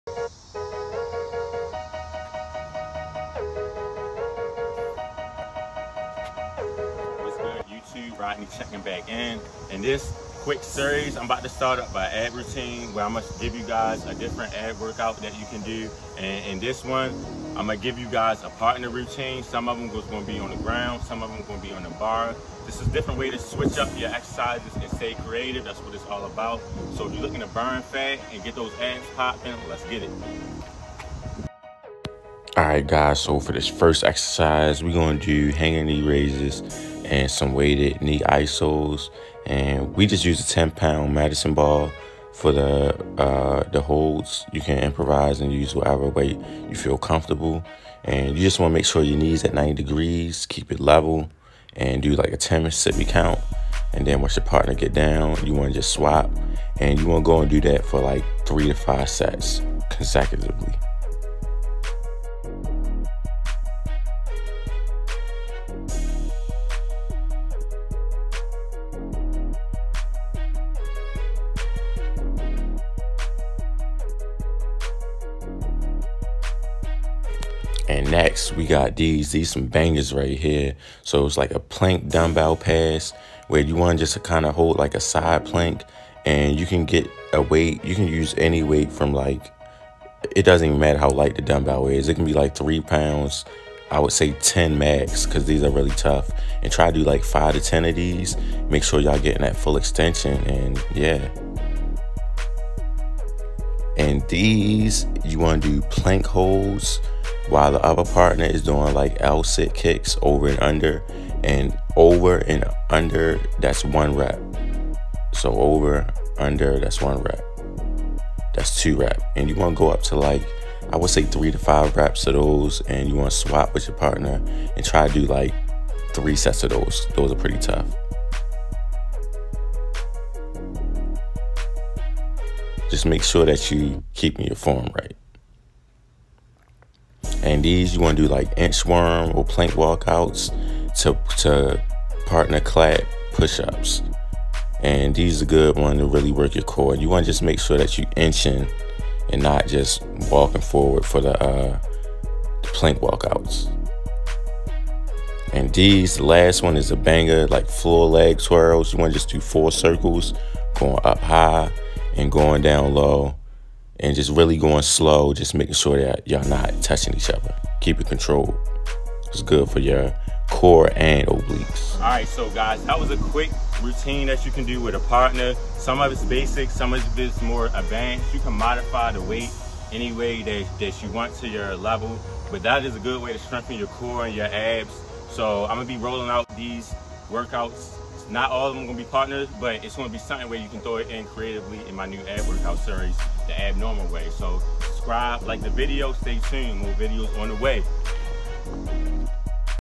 ABnormal AB CORE Workout Routine ( Calisthenics/Bodyweight)
First workout of my Ab core workout routine. Great for shredding, cutting body fat & improving core strength. Grab a partner & GET BUSY‼️